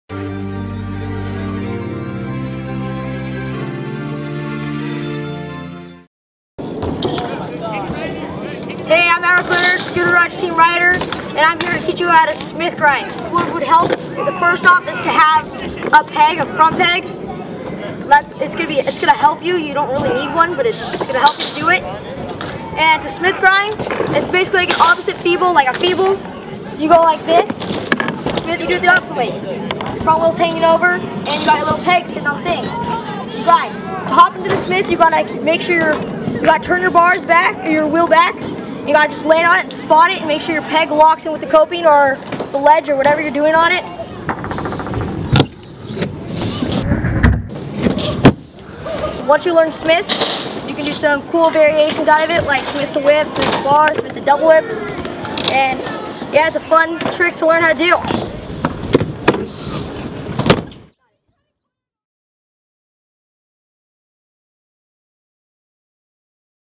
How to Smith Grind with Maverick Leonard
Maverick Leonard teaches you how to smith grind.